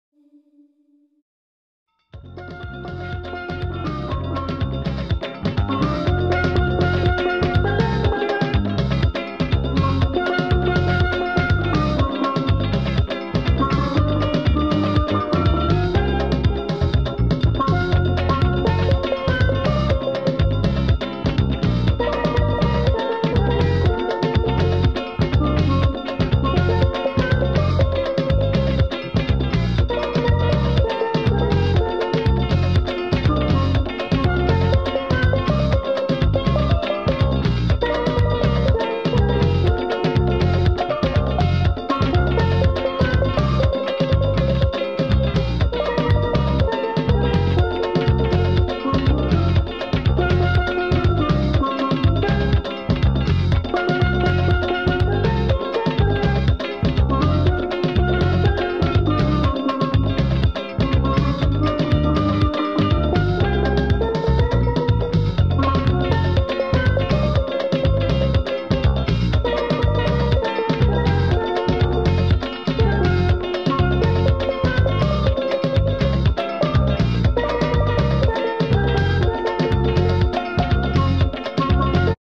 Mandalay visits the Island of Bequia
When sailing on the Mandalay this is one of the Islands you will be visiting.